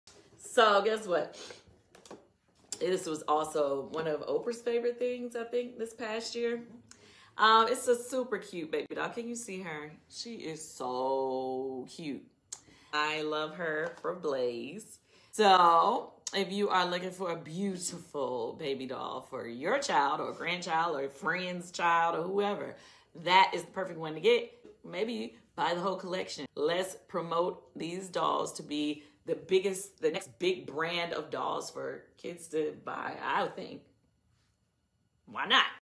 Kandi Burrass highlights Orijin Bees in Amazon Livestream
Shop our Baby Bee Collection of dolls in various skin tones, hair colors and curl patterns from 4c-3c!!